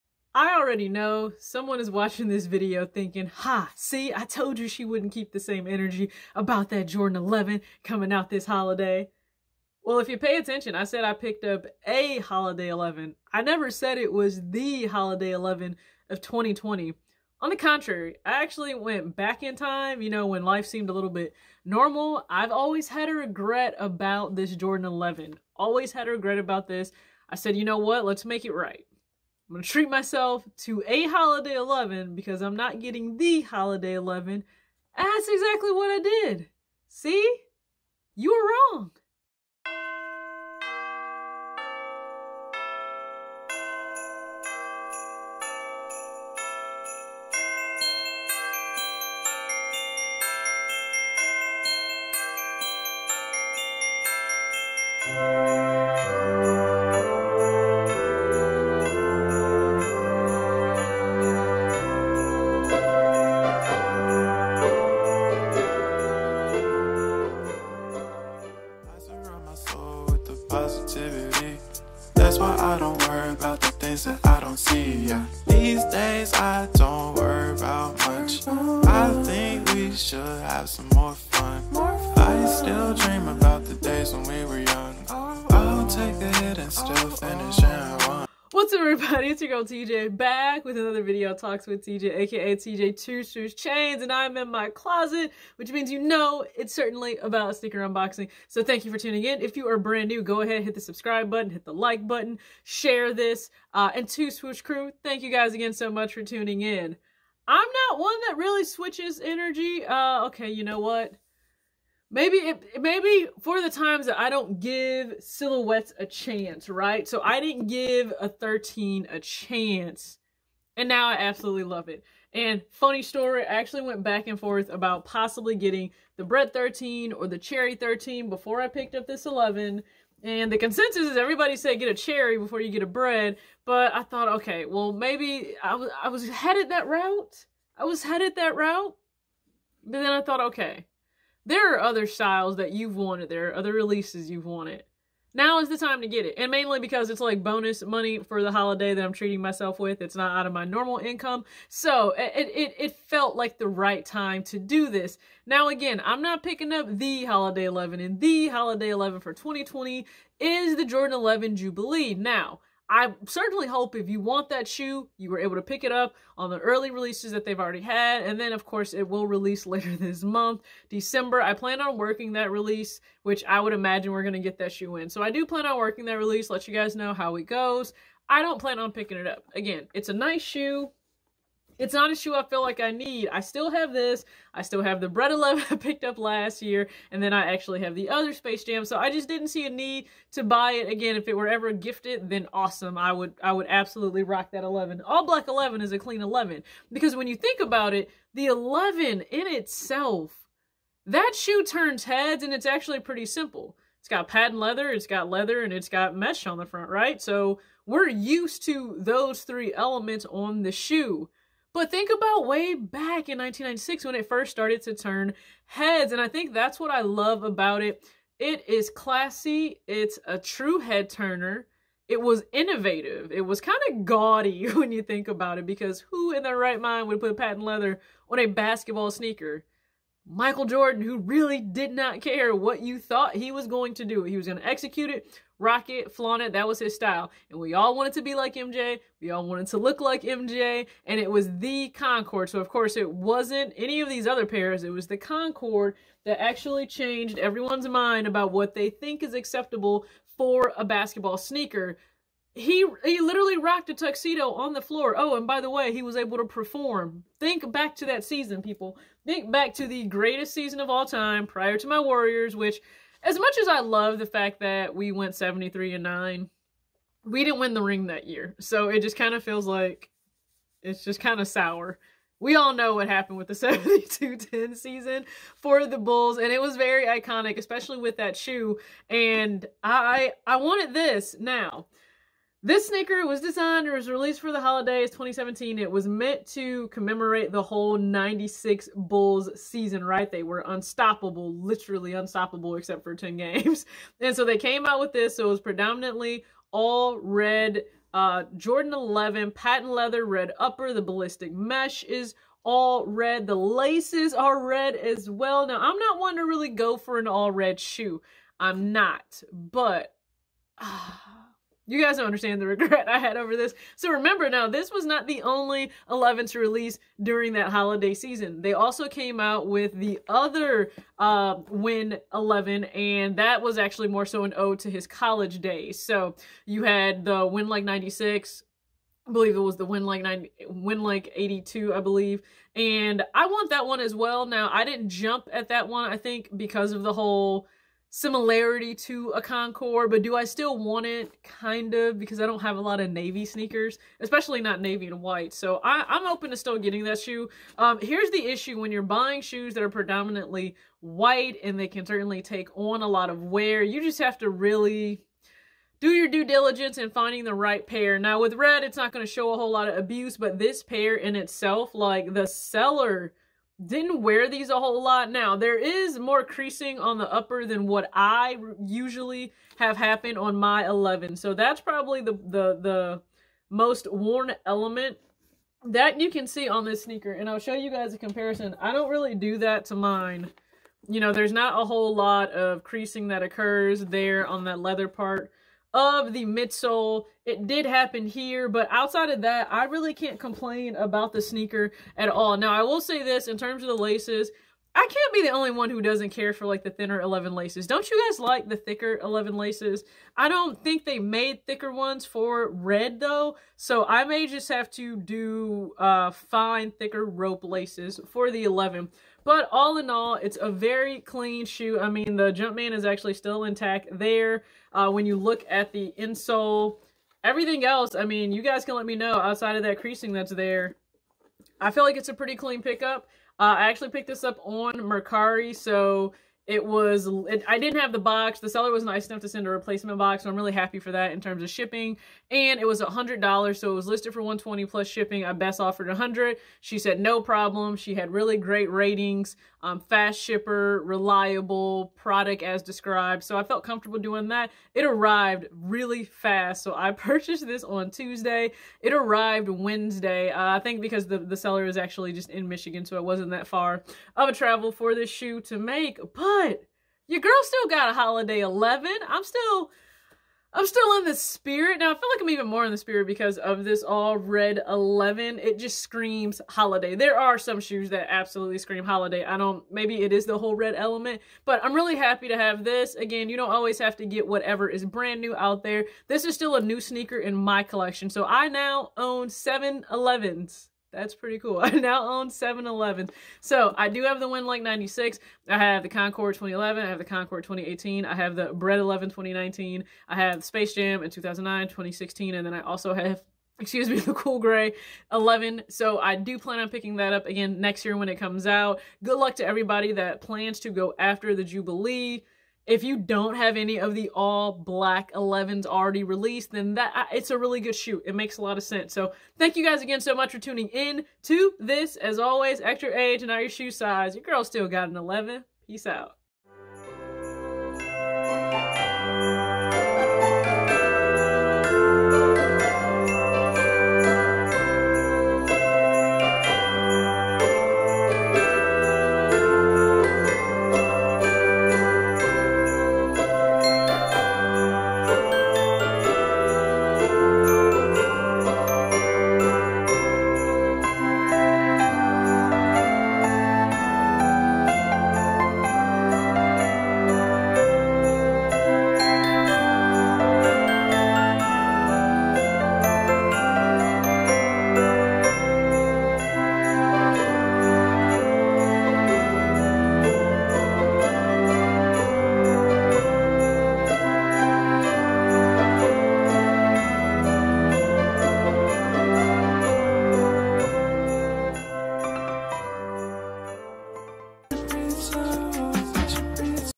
So...I Copped a Air Jordan XI for Holiday 2020...
Yes, no cap I bought a jordan11 for holiday2020 because I love sneakers oh, did you assume what Jordan XI I bought? Well that's on you isn't lol, hopefully you enjoyed this review and good luck to everyone going after the Jordan XI Jubilee next week, December 12th.

Musician: Rook1e

Google Pixel 4XL or Samsung Galaxy S10E – All pics on my IG page are shot with the same as well.